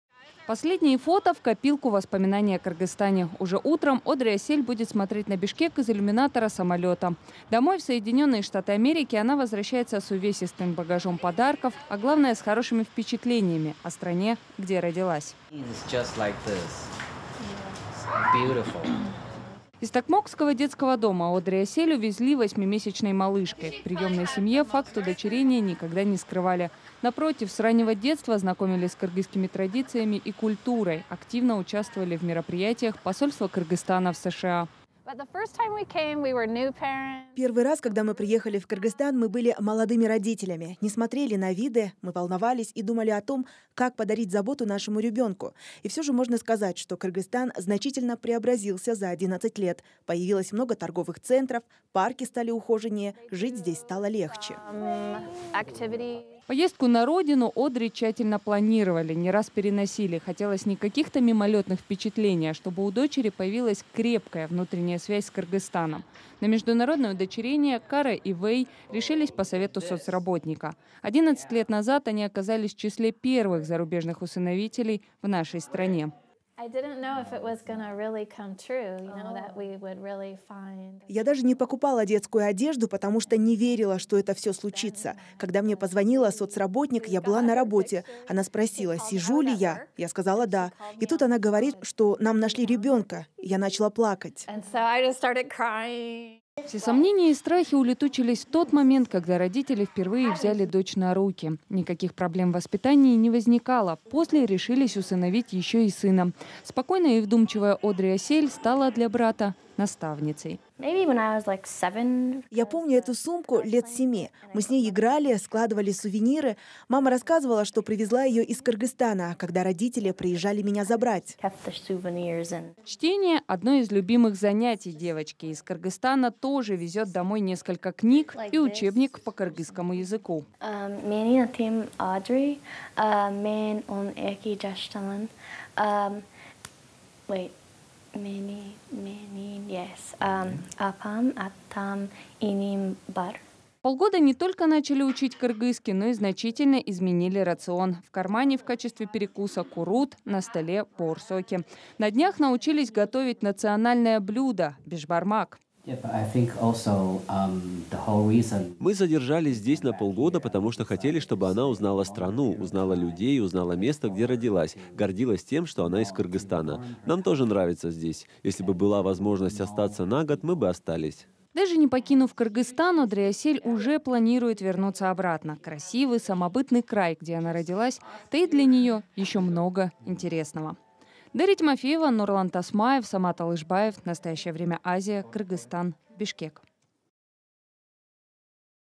Одри Асель побывала в Бишкеке. История кыргызской девочки, выросшей в США
12-летняя кыргызская девочка Одри Асель была удочерена 11 лет назад. Все детство она провела в Соединенных Штатах Америки. И вот за последние полгода она познакомилась с кыргызским языком и узнала места, которые раньше видела только на фото. Сюжет "Настоящего времени".